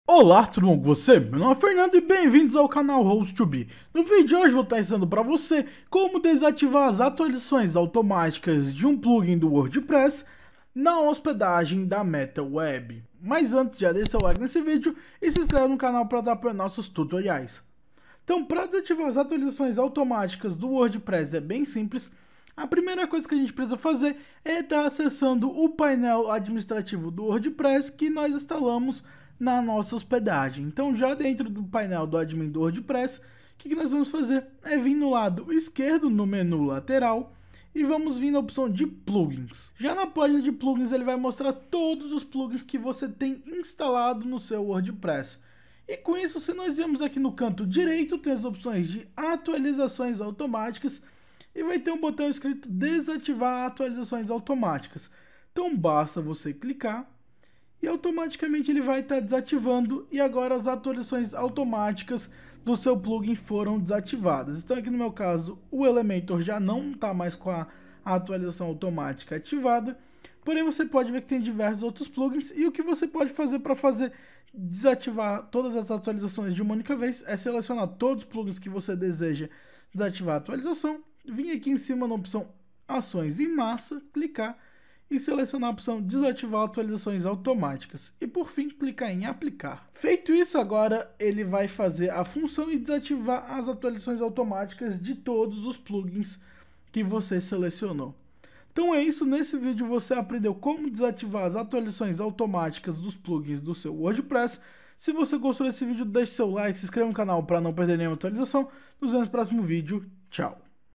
Como Desativar as Atualizações Automaticas de Plugins do WordPress na Metaweb (Rápido e Fácil) 2022
Aprenda Como Desativar as Atualizações Automaticas de Plugins do WordPress na Metaweb facilmente e em poucos minutos